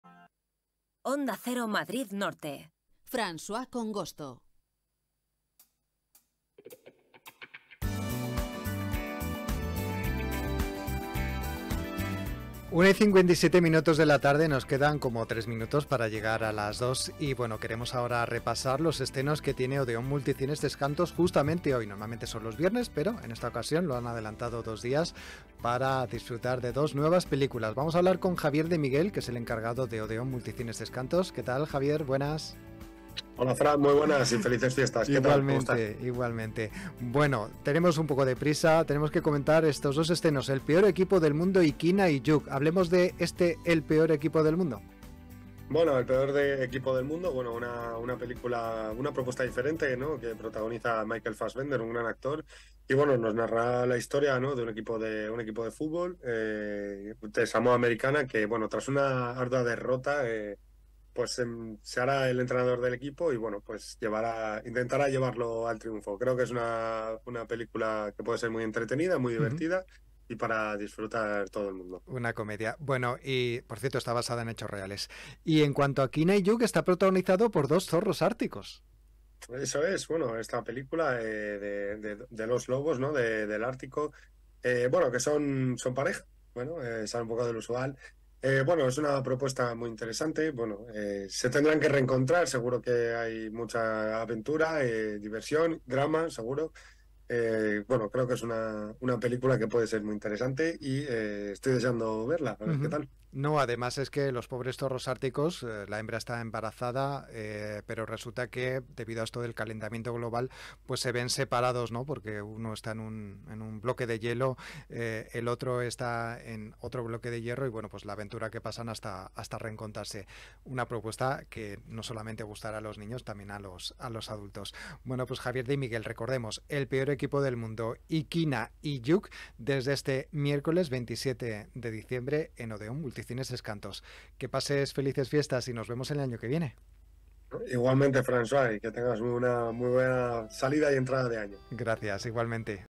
Estrenos de cine Odeón Multicines Tres Cantos
Terminaremos el programa conociendo los estrenos de cine que llegan este miércoles a Odeón Multicines Tres Cantos.